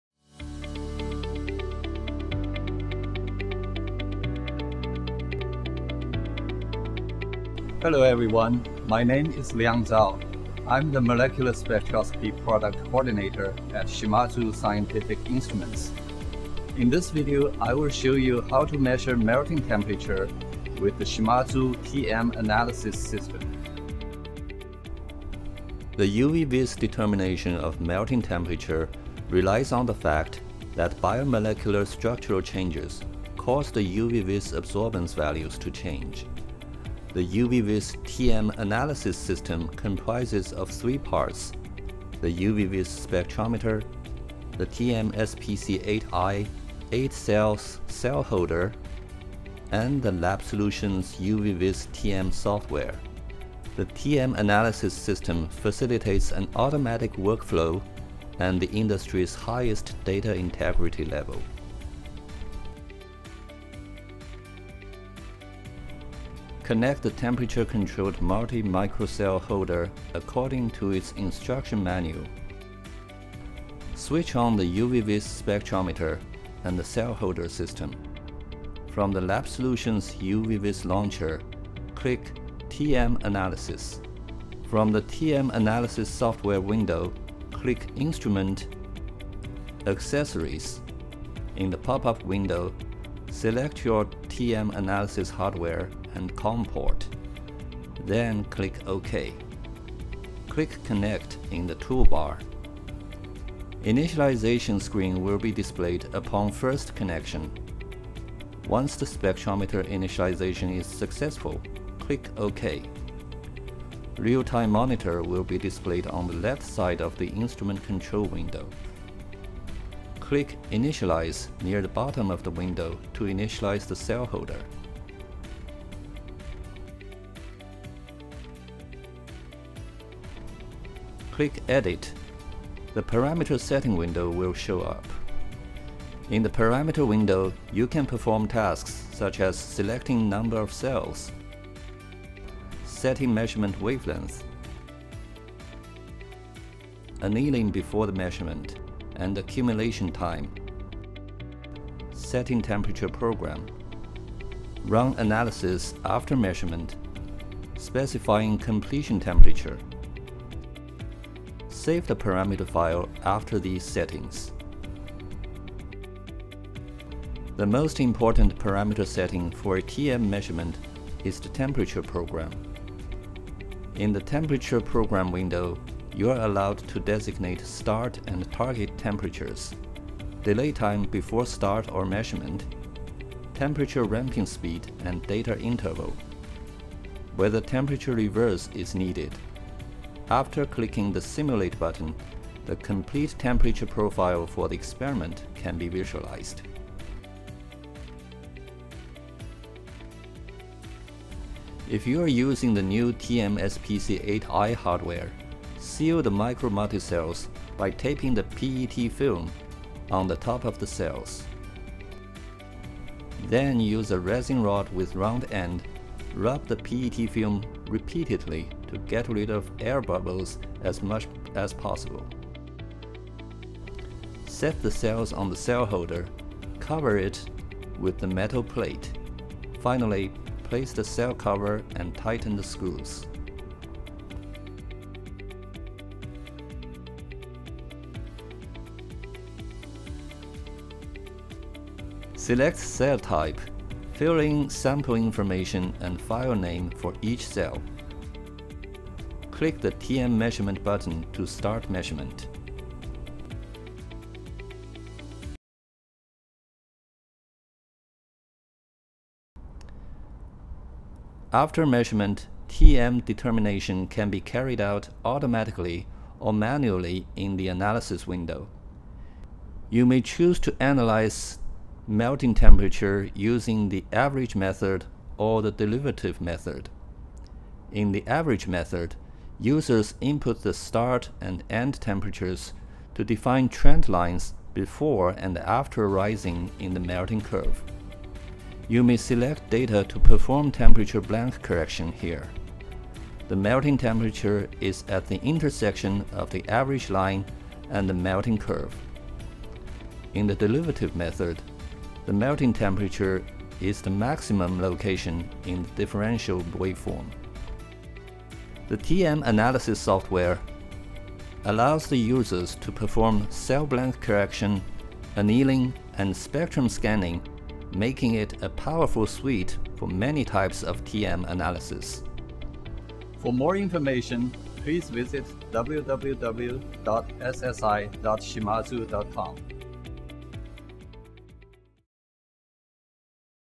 How to Measure Melting Temperature using the Shimadzu TM Analysis System
How to Measure Melting Temperature using the TM Analysis System

Shimadzu’s new Tm Analysis system comprises of the UV-Vis spectrometer, the TMSPC8i temperature-controlled cell holder, and the LabSolutions UV-Vis Tm Analysis software. Determination of melting temperature for your biomolecules can be achieved with great flexibility and efficiency. Annealing, blank correction, temperature reverse, spectrum acquisition of samples can be finished within the Tm Analysis software. The new sealing method on the TMSPC-8i hardware almost get rid of sample evaporation near boiling temperature. Optional LabSolutions DB/CS software enables compliance with ER/ES-related regulatory requirements. Overall, this is an amazing tool for your oligonucleotide/peptide therapeutic research.

0:00 - Intro
1:03 - Connect Hardware and Establish Connection
2:07 - Set Parameters
3:18 - Seal and Load Cells
3:58 – Start Measurement
4:16 - Tm Analysis
5:08 – Conclusion